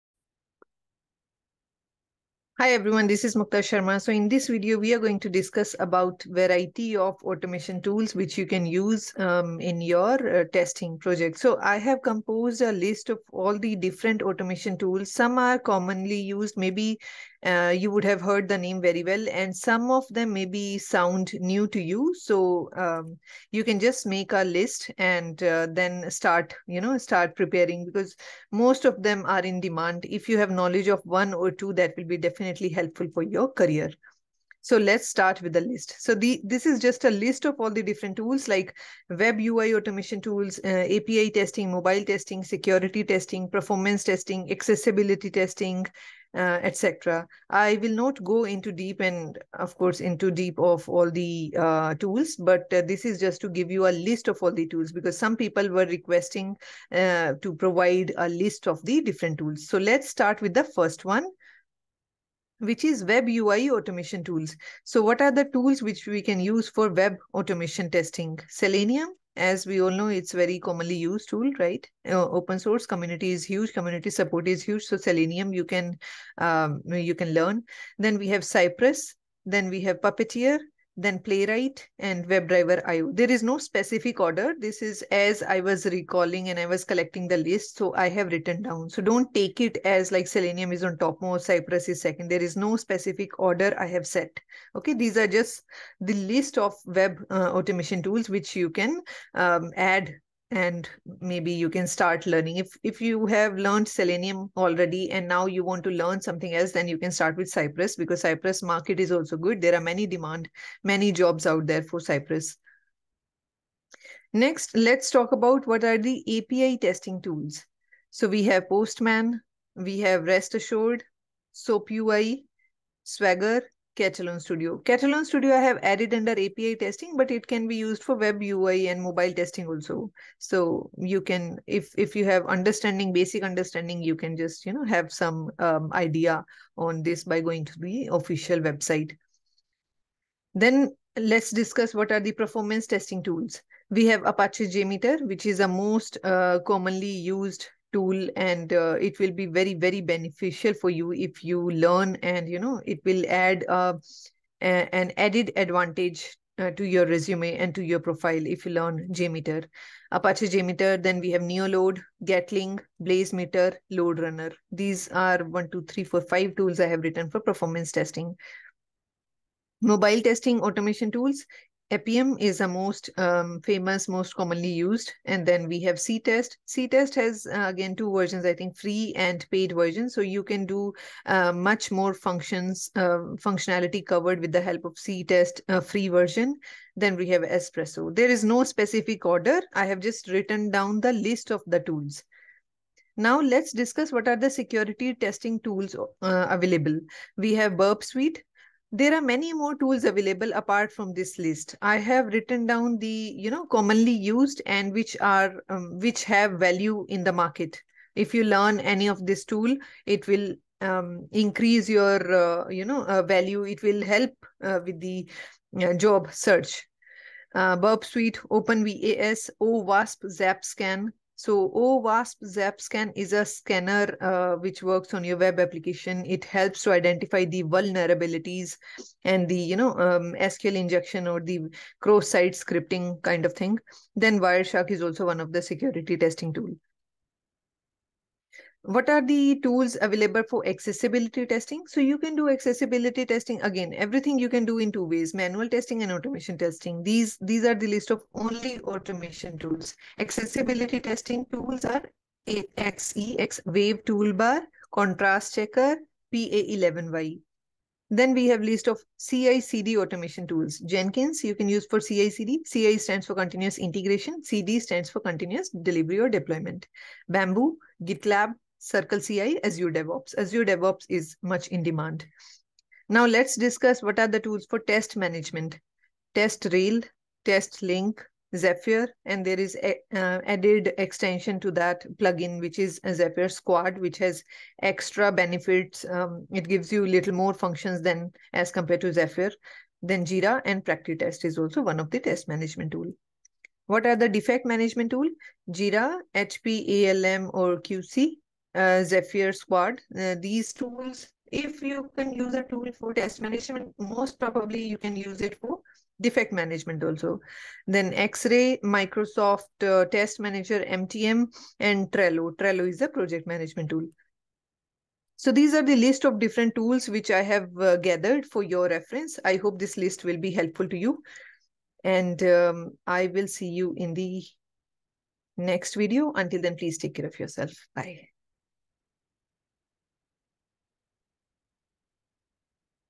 List of all different automation tools in testing. -All famous commonly used automation tools
Hi All, In this video, i am providing you a list of all different automation tools for a software testing professional. Thank you!